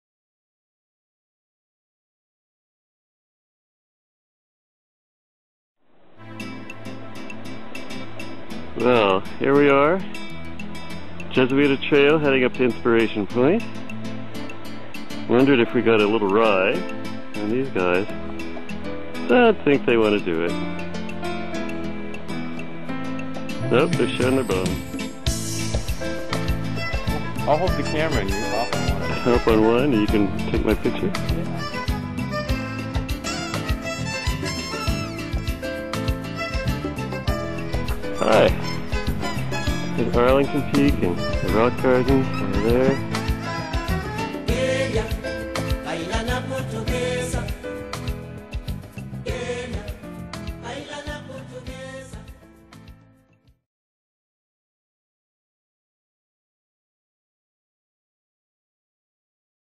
Jesuita Trail
Jesuita Trail up to Inspiration Point, with local fauna.